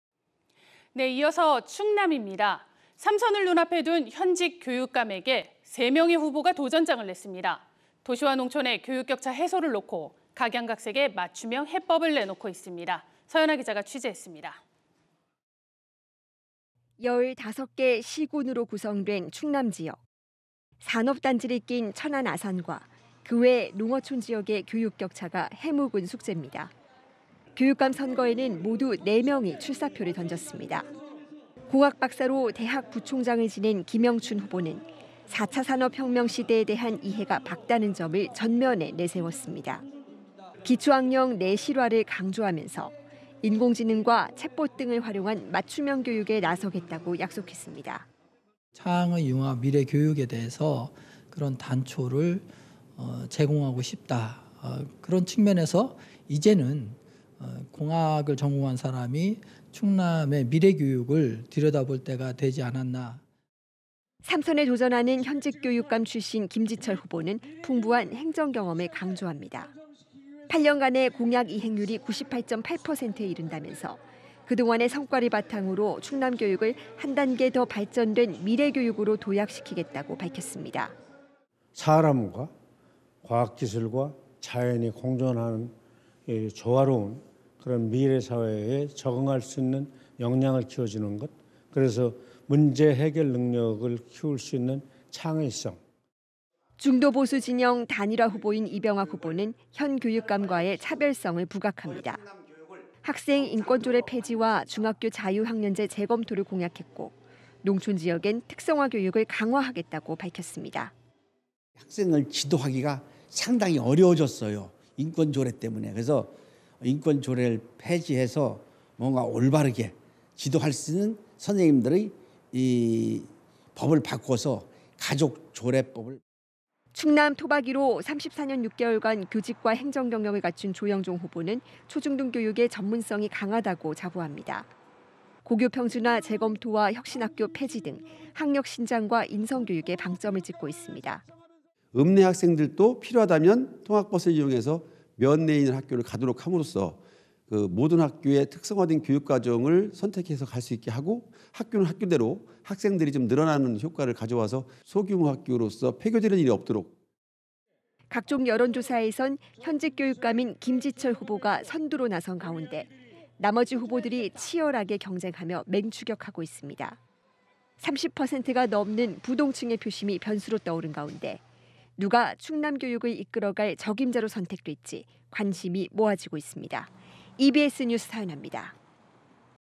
2022. 05. 26. 현직 교육감에 3명 도전장 ‥'도농 격차·미래교육' 화두
[EBS 뉴스]

이번엔 충남으로 가봅니다.

3선을 눈앞에 둔 현직 교육감에게 세 명의 후보가 도전장을 냈습니다.

도시와 농촌의 교육격차 해소를 놓고, 각양각색의 맞춤형 해법을 내놓고 있습니다.

서현아 기자가 취재했습니다.

[리포트]

15개 시군으로 구성된 충남 지역.

산업 단지를 낀 천안 아산과 그 외 농어촌 지역의 교육격차가 해묵은 숙제입니다.

교육감 선거에는 모두 네 명이 출사표를 던졌습니다.

공학 박사로 대학 부총장을 지낸 김영춘 후보는 4차 산업 혁명 시대에 대한 이해가 밝다는 점을 전면에 내세웠습니다.

기초학력 내실화를 강조하면서, 인공지능과 챗봇 등을 활용한 맞춤형 교육에 나서겠다고 약속했습니다.

인터뷰: 김영춘 / 충남교육감 후보
"창의융합미래교육에 대해서 그런 단초를 제공하고 싶다, 그런 측면에서 이제는 공학을 전공한 사람이 충남의 미래교육을 들여다볼 때가 되지 않았나…"

3선에 도전하는 현직 교육감 김지철 후보는 풍부한 행정 경험을 강조합니다.

8년간의 공약 이행률이 98.8%에 이른다면서, 그동안의 성과를 바탕으로 충남교육을 한 단계 더 발전된 미래 교육으로 도약시키겠다는 각오를 다졌습니다.

인터뷰: 김지철 / 충남교육감 후보
"사람과 과학기술과 자연이 공존하는 조화로운 미래사회에 적응할 수 있는 역량을 키워주는 것, 그래서 문제해결 능력을 키울 수 있는 창의성을…"

중도보수 진영 단일화 후보인 이병학 후보는 현 교육감과의 차별성을 부각합니다.

학생인권조례 폐지와 중학교 자유학년제 재검토를 공약했고, 농촌 지역엔 특성화 교육을 강화하겠다고 밝혔습니다.

인터뷰: 이병학 / 충남교육감 후보
"학생을 지도하기가 상당히 어려워졌어요, 인권조례 떄문에. 그래서 인권조례를 폐지해서 올바르게 지도할 수 있는 선생님들을 법을 바꿔서 가족조례법을…"

충남 토박이로 34년 6개월간 교직과 행정 경력을 갖춘 조영종 후보는 초중등 교육에 전문성이 강하다고 자부합니다.

고교평준화 재검토와 혁신학교 폐지 등, 학력 신장과 인성교육에 방점을 찍고 있습니다.

인터뷰: 조영종 / 충남교육감 후보
"읍내 학생들도 필요하다면 통학버스를 이용해서 면내에 있는 학교를 가게 함으로써 모든 학교에 특성화된 교육과정을 선택해서 갈 수 있도록 하고 학교는 학교대로 학생들이 늘어날 수 있는 효과를 가져와서 소규모 학교가 폐교되는 일이 없도록"

각종 여론조사에선 현직 교육감인 김지철 후보가 선두로 나선 가운데, 나머지 후보들이 치열하게 경쟁하며 맹추격하고 있습니다.

30%가 넘는 부동층의 표심이 변수로 떠오른 가운데, 누가 충남교육을 이끌어갈 적임자로 선택될지, 관심이 모아지고 있습니다.

EBS 뉴스, 서현아입니다.